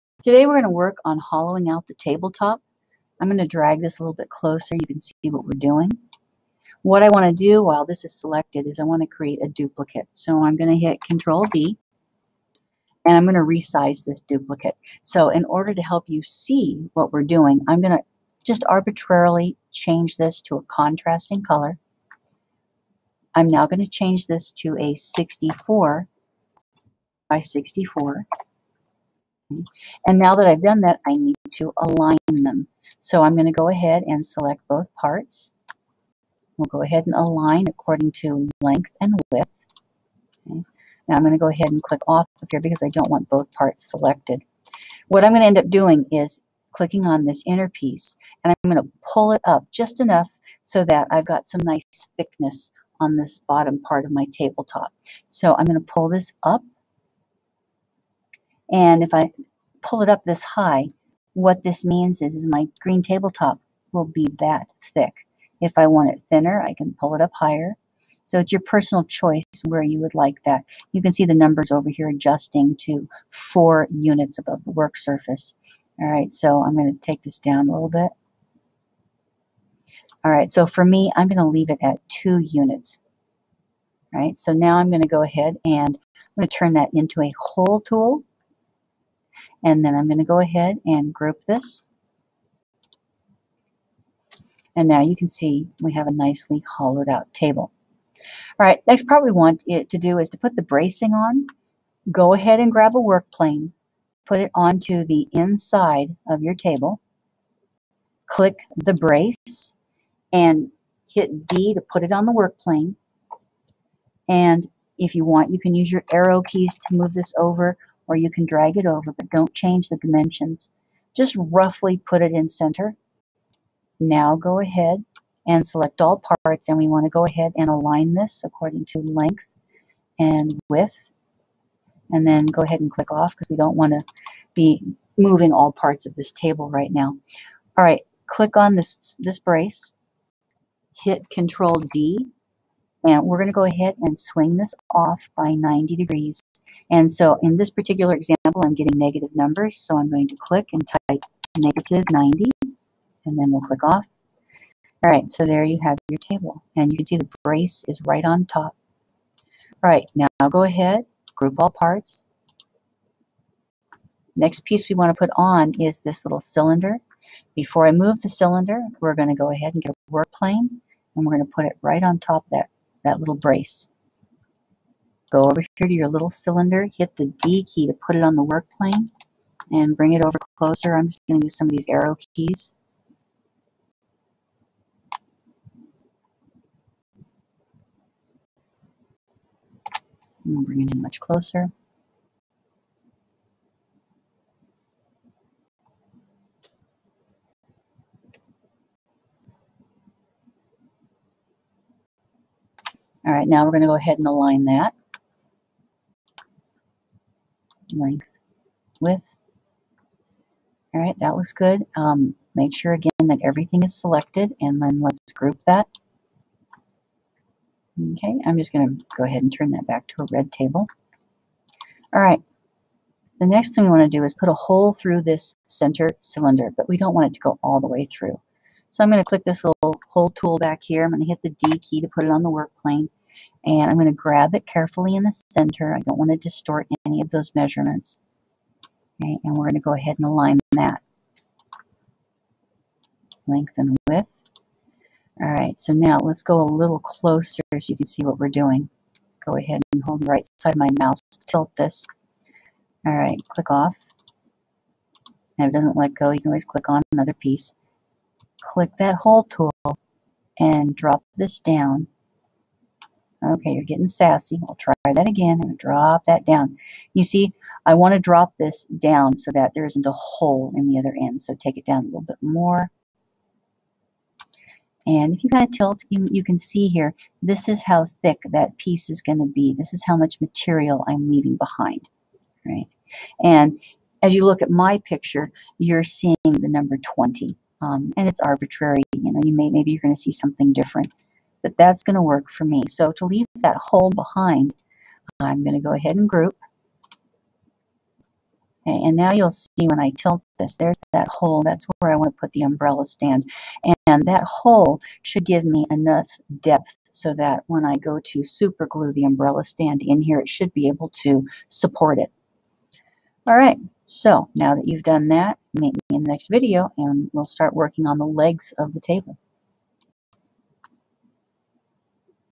2) Patio Table (underside), TinkerCad Tutorial
In this video, we'll duplicate and hollow out the table top.  I duplicated the table to 64 x 64 x 5.5. Once hollowed out, remember to use the workplane, placing it onto the underside of the table and then adding a brace. The brace is then duplicated and angled to 90 degrees. Next, the cylinder is added. The final step shows how to combine the hole cylinder to remove some material for an eventual umbrella stand.